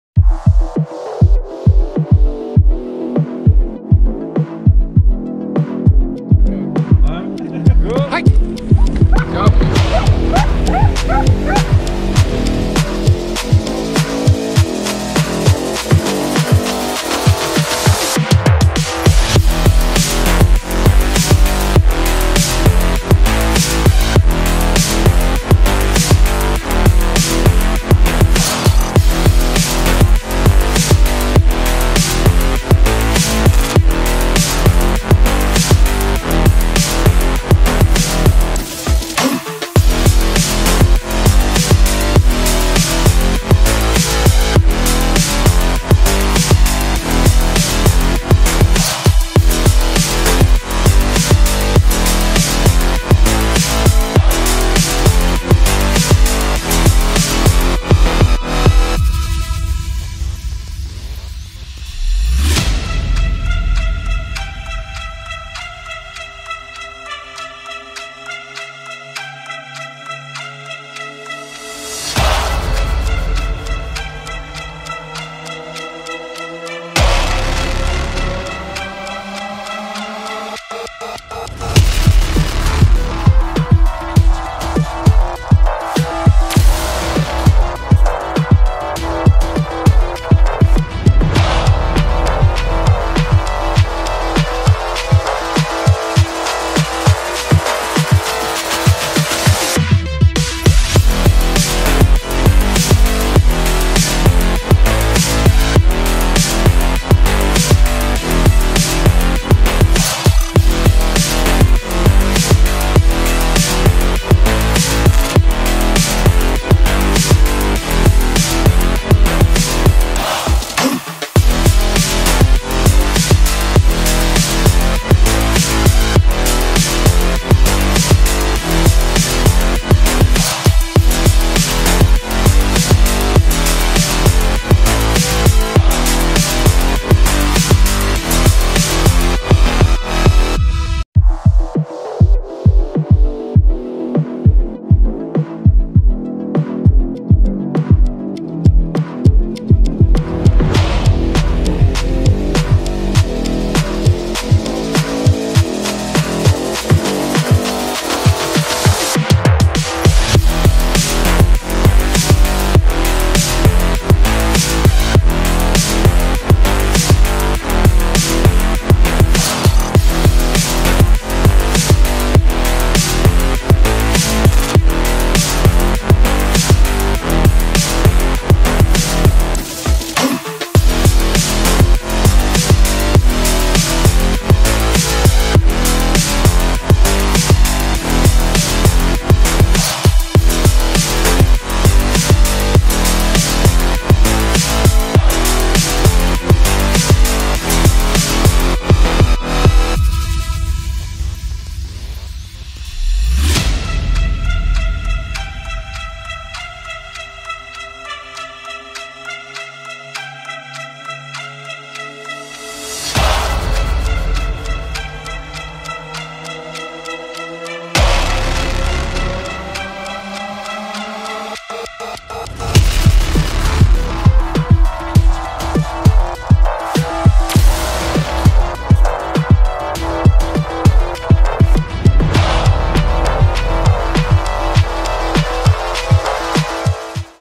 Race Video - Kearney Dog Sled Races 2023 - 2-dog sled class 2 miles
Kearney Dog Sled Races 2023 - 2-dog Sled class 2 miles. Finished in 2nd position with only one dog!